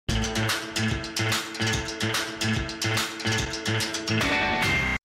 A&E - Classificação indicativa 18 anos (2017) [Full HD]
Esta versão não é tão comum às 7 da noite (UTC -3). Mas foi exibida antes de «Robocop».
Parte do pacote gráfico estreado em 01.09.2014 e substituído em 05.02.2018.